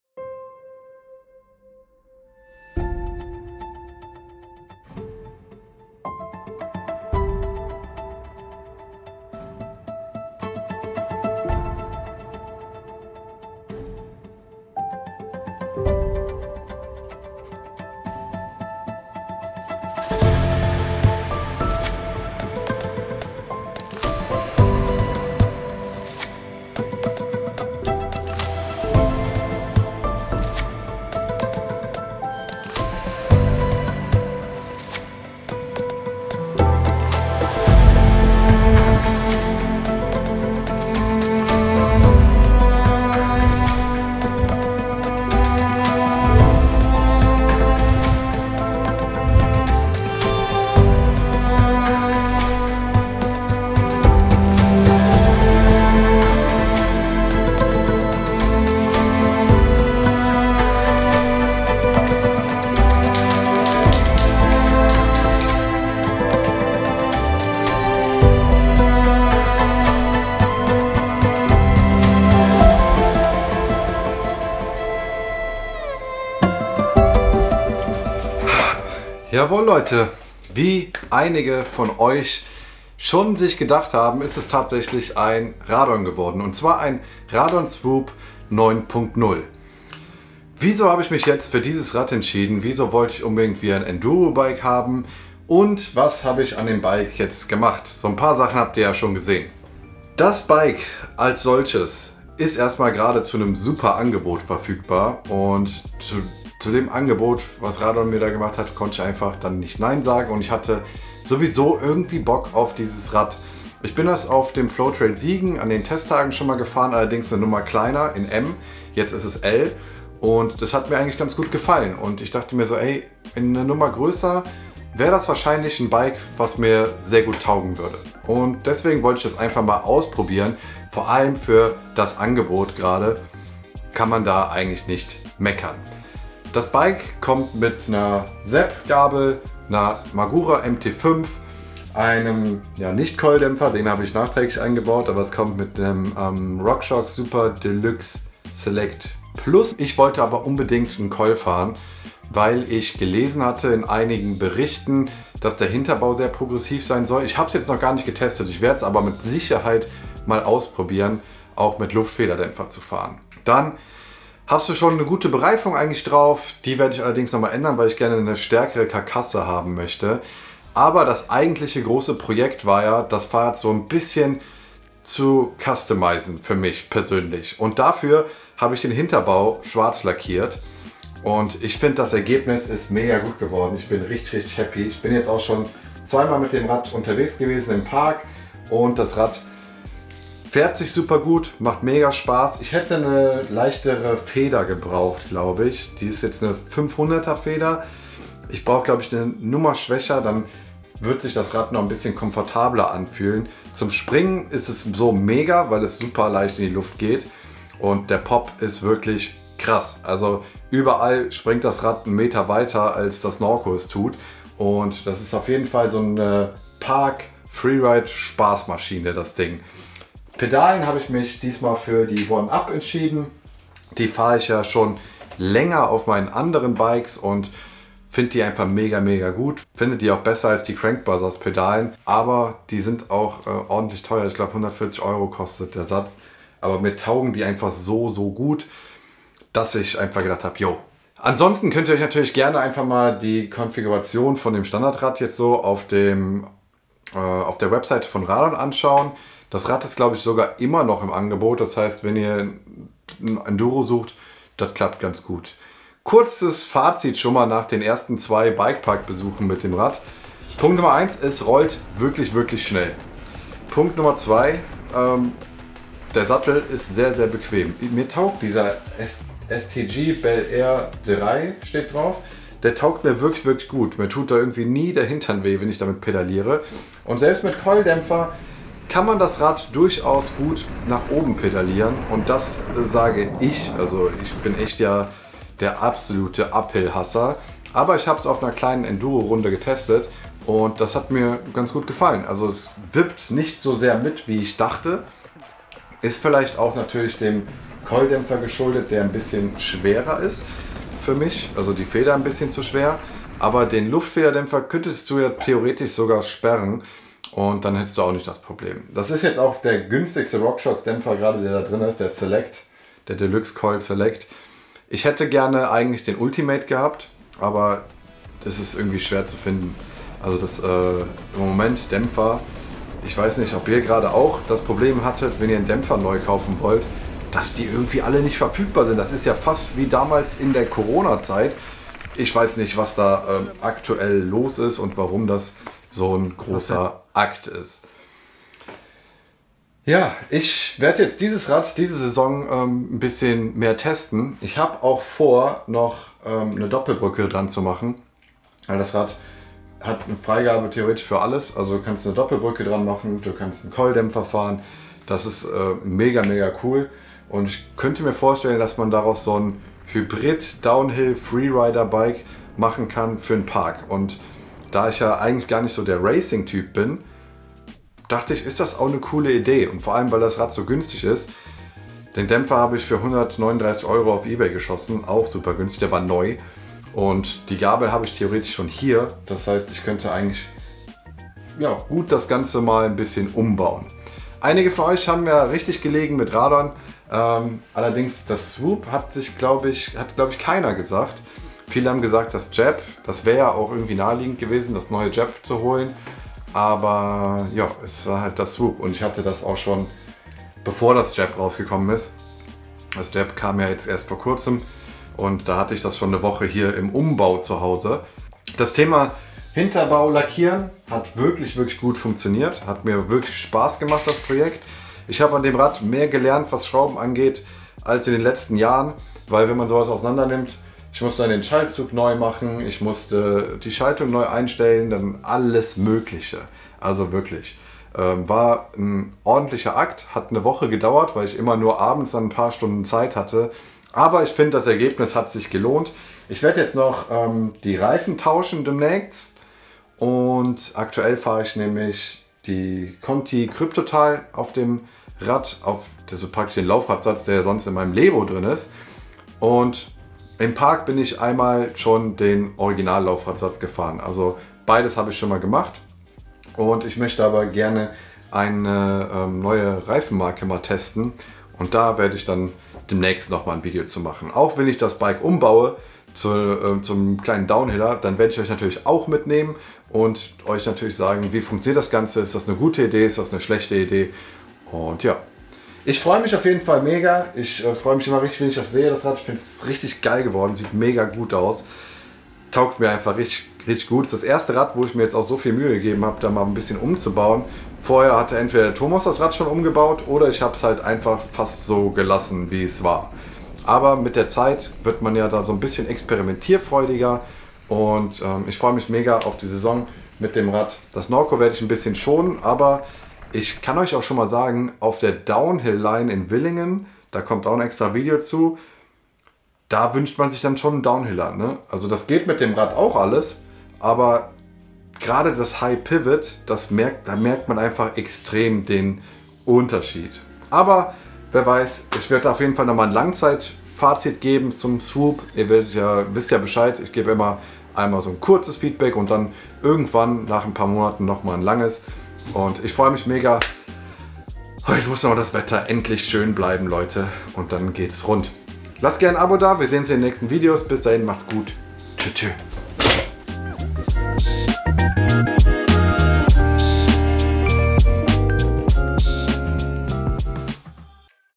NEW BIKE DAY I Enduro Bike Comeback I Radon Swoop 9.0 Custom
Ich habe ein neues Bike für 2024 und zwar das Radon Swoop 9.0 ich habe allerdings so manches verändert wie einen Coil Dämpfer und habe einen Teil vom Mountainbike Rahmen selbst lackiert um es etwas mir individuell anzupassen!

►►► MTB Ausrüstung

DH Helm
Rücken/Brust Protektor
Reinigung Tools

Main Camera
Sek. Camera
Funk Mic
FPV Drohne

MacBook für Editing
Mac Studio